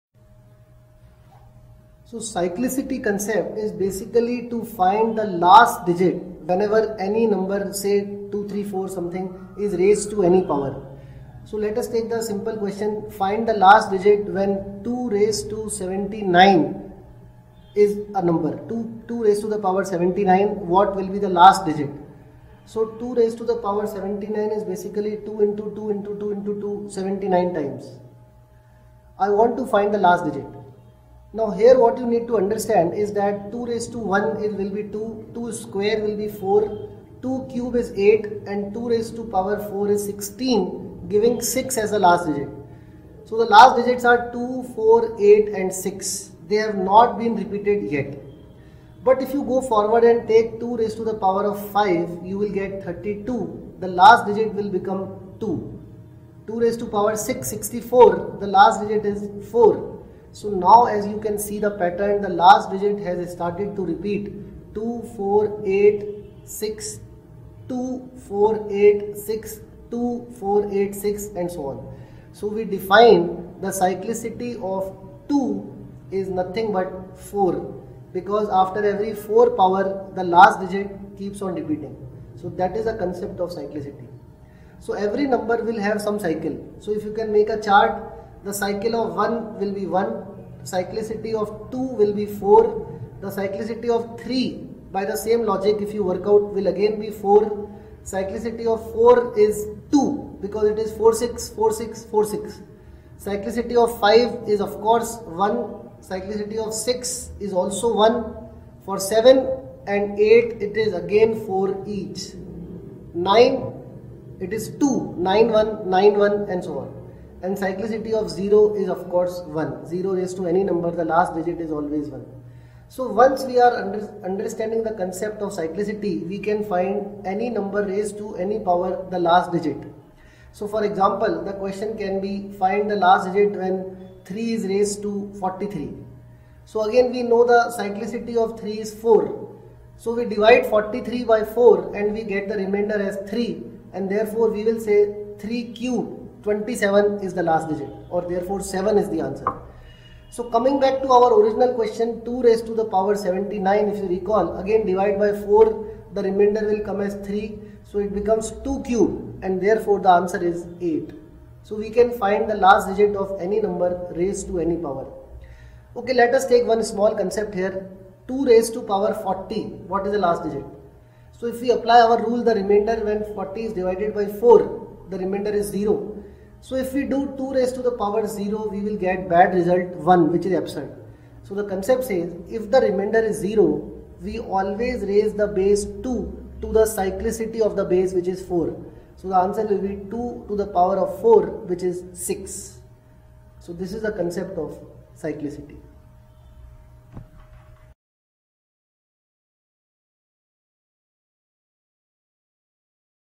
Cyclicity Concept
Explains concept of Cyclicity along with use of cyclicity in problems